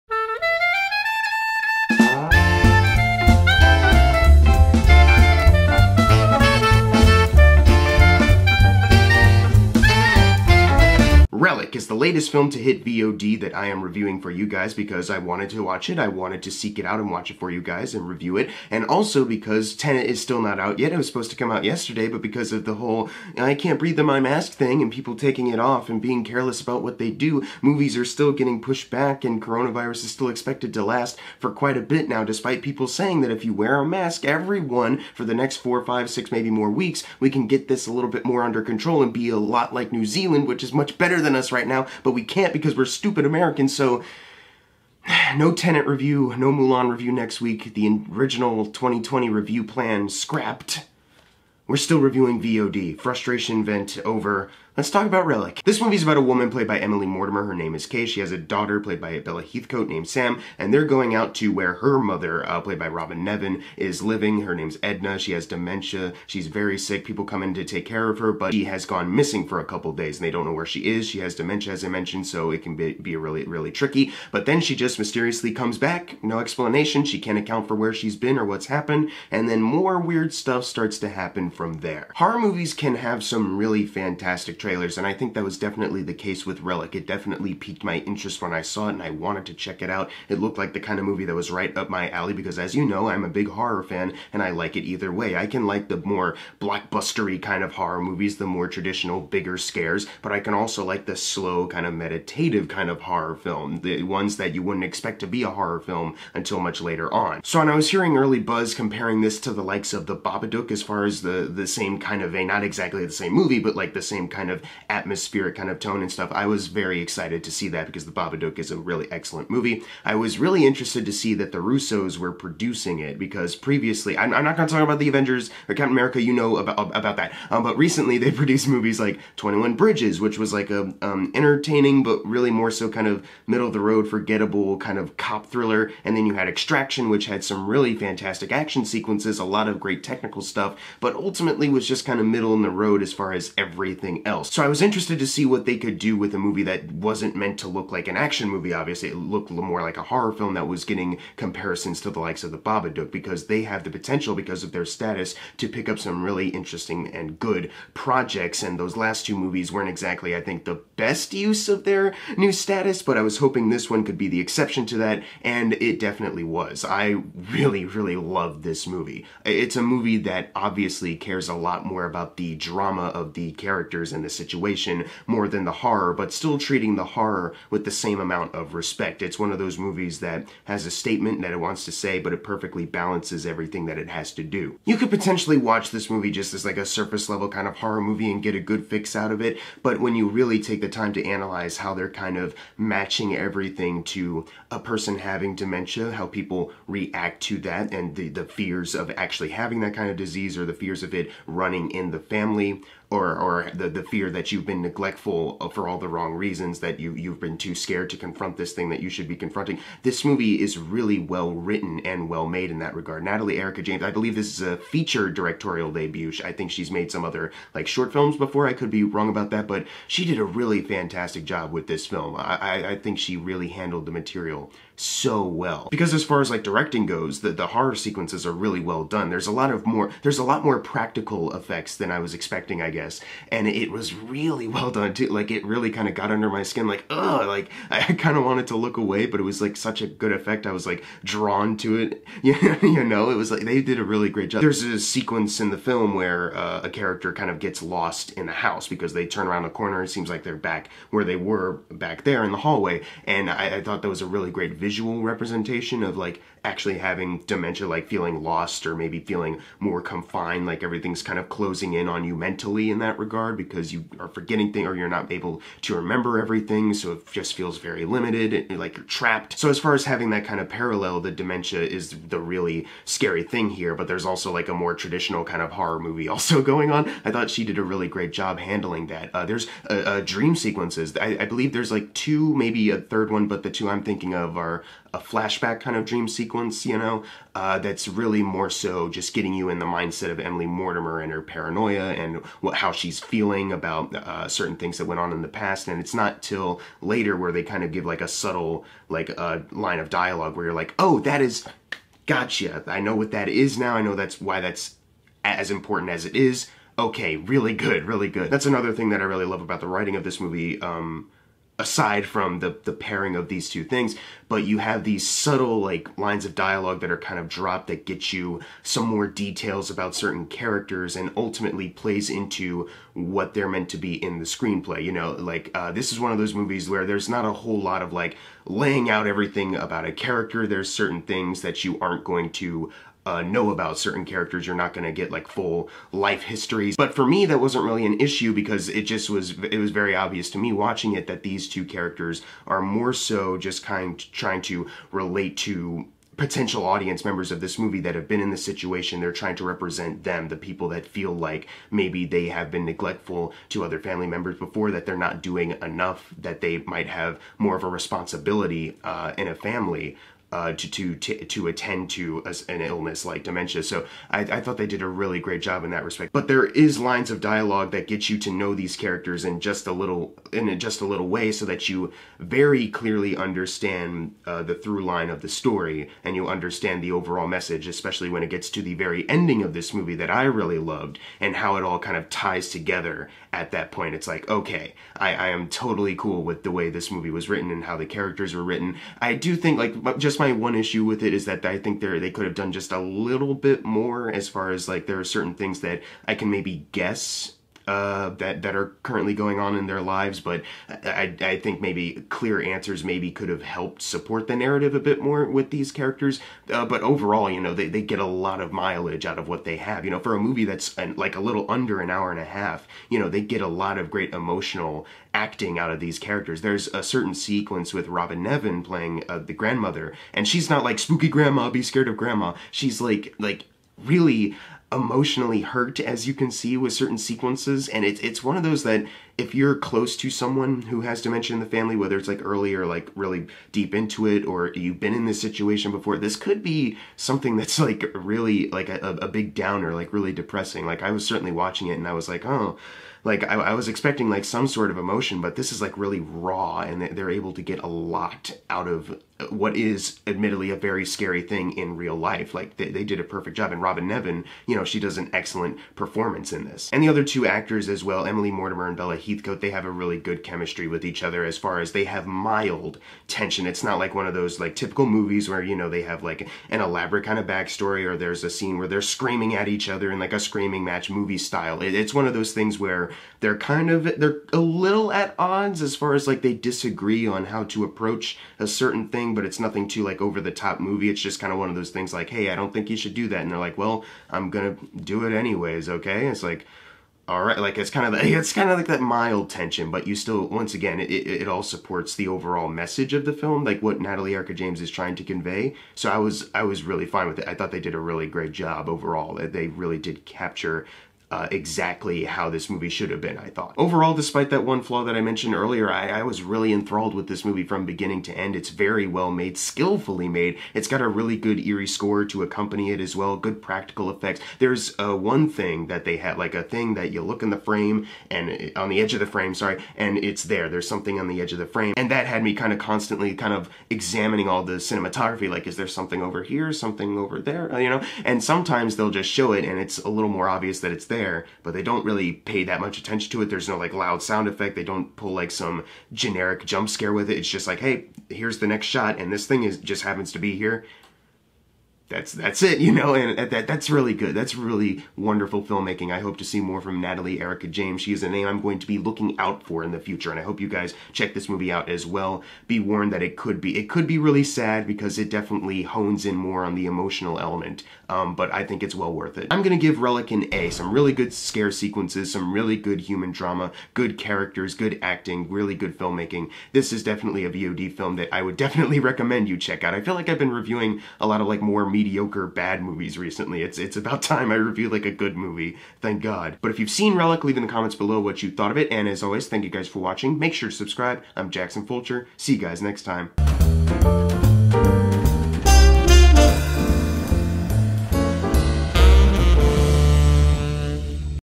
Relic - Movie Review
Can Relic earn an emotional reaction from its audience as well as being legitimately creepy? Here's my review!